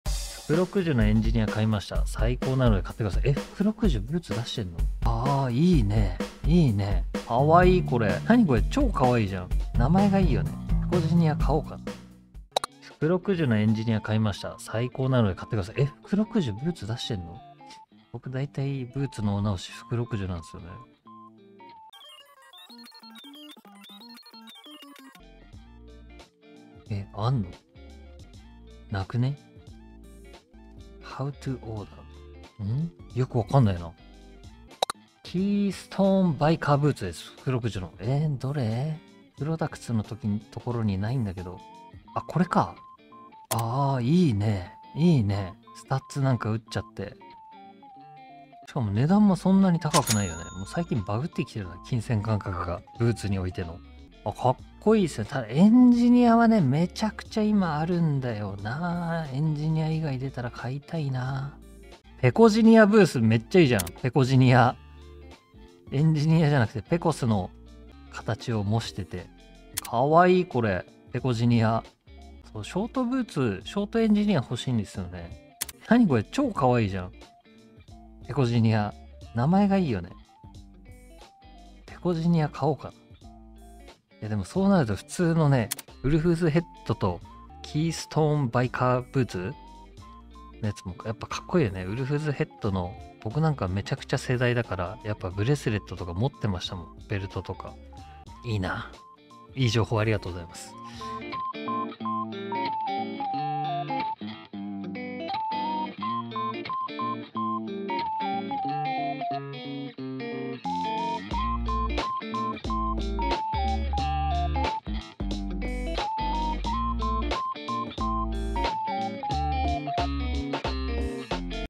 【福禄寿】ペコス×エンジニア!? ペコジニアブーツが可愛すぎる！！【Takahiro Kawashima切り抜き】
ショートブーツっていい！
いつか行きたい、福禄寿。

紹介商品
福禄寿
・KEYSTONE PECOGINEER BLACK

・WOLF’S HEAD × KEYSTONE STUDS BIKER BOOTS – SHAFT:BLACK VAMP:BLACK STUDS:SILVER